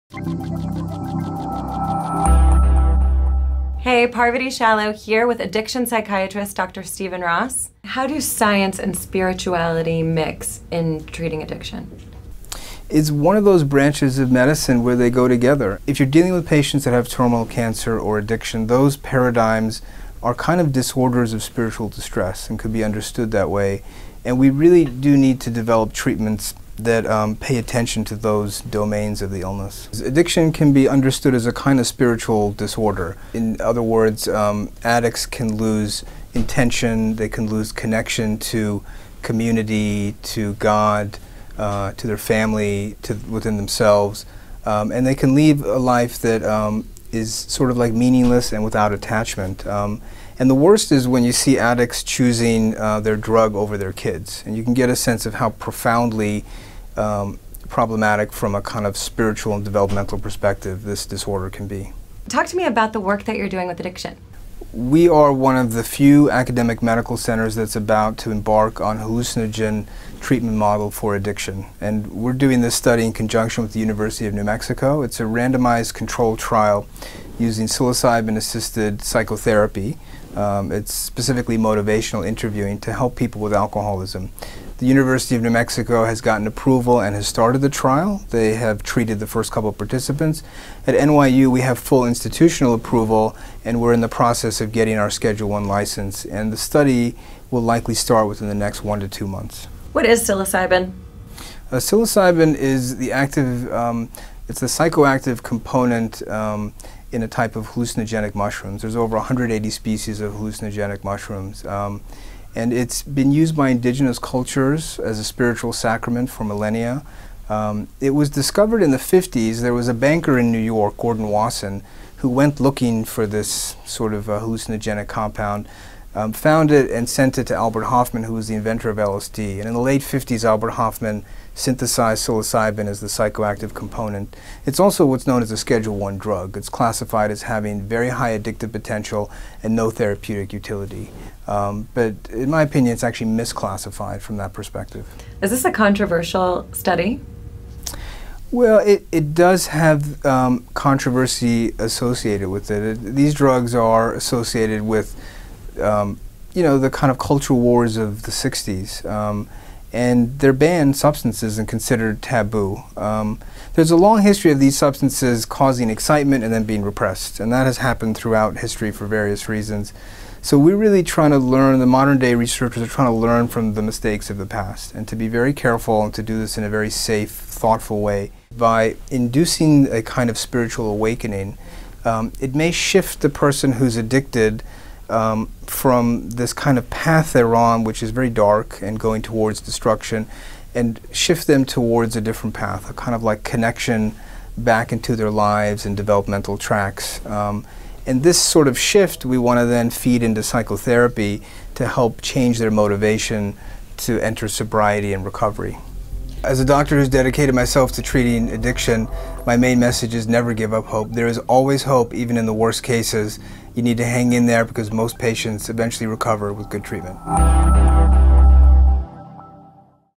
Science, spirituality, and psilocybin
Dr. Stephen Ross, director of the NYU Addiction Psychiatry Fellowship, talks about his plans for a new study on how a hallucinogenic compound, psilocybin, might help those with alcoholism.